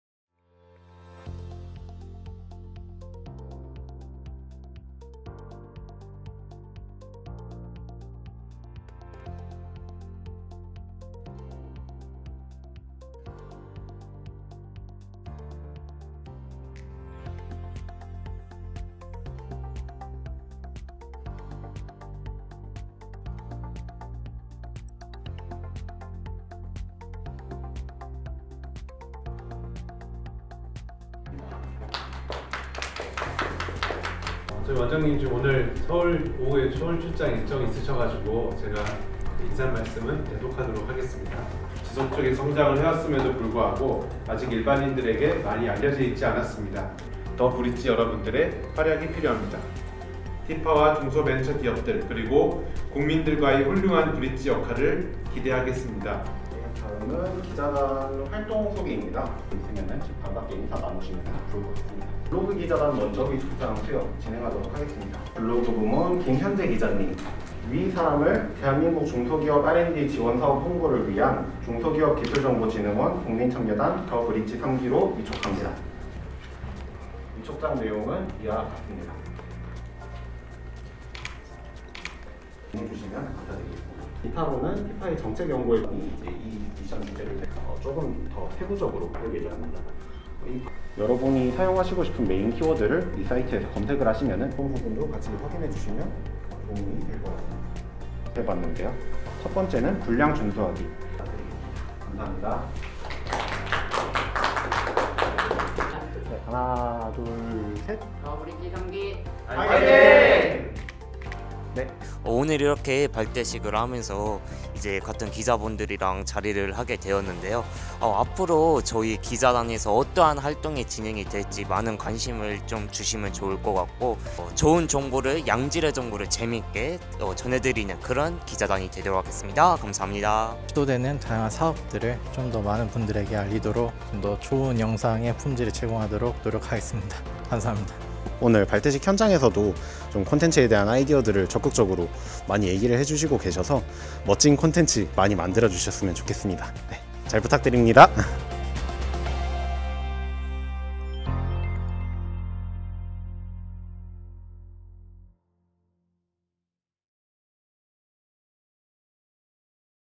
[The BRIDGE 3기] The BRIDGE 3기 발대식 현장 스케치 _ 이종진 기자
[TIPA 국민참여기자단 The BRIDGE 3기 – 이종진 기자]

The BRIDGE 3기 발대식 현장 스케치!

TIPA 국민참여기자단 The BRIDGE 3기 이종진 기자님이 취재한 The BRIDGE 3기의 발대식,
The BRIDGE 3기의 첫 만남을 영상을 통해 만나보세요!

중소기업기술정보진흥원을 구독하세요!😁😄

[중소기업기술정보진흥원 SNS & 페이지]